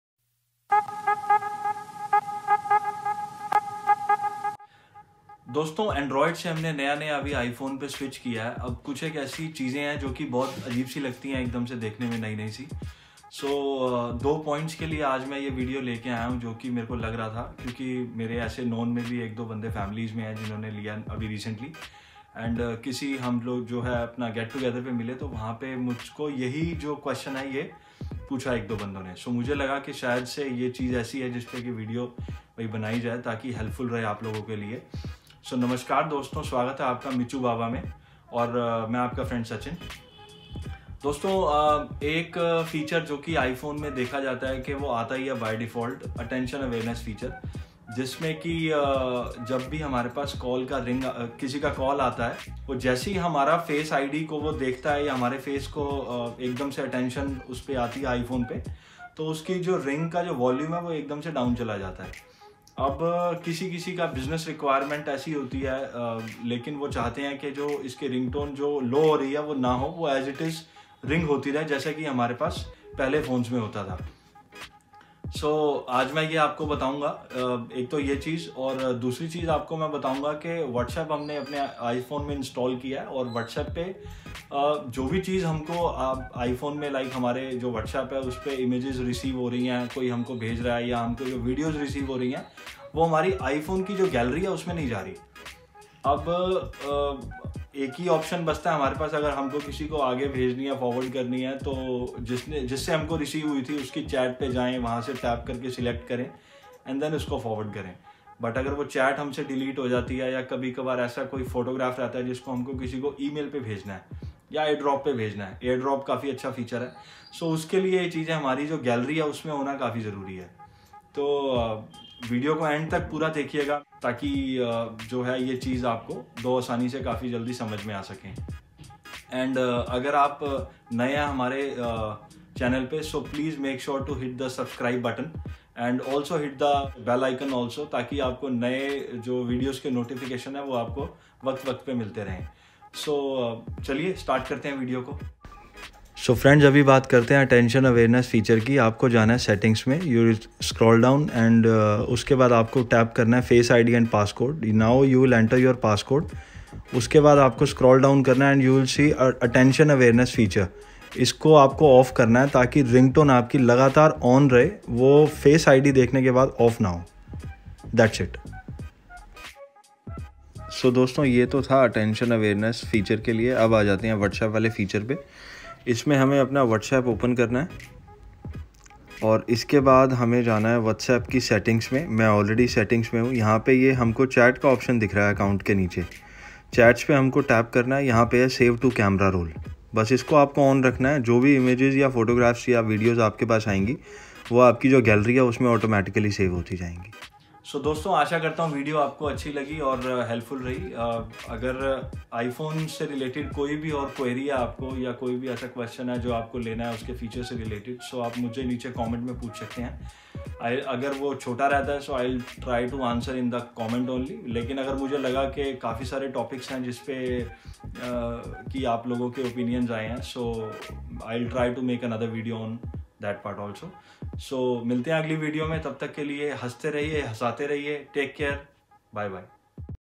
Whatsapp images not showing in iphone gallery | Ring volume goes down
Somebody who has just switched from an android to an ios device may be looking for these options and find this video helpful.

Whatsapp images are not getting saved to my iphone gallery

My iPhone ringtone volume goes down whenever someone calls me and I pick up the phone to answer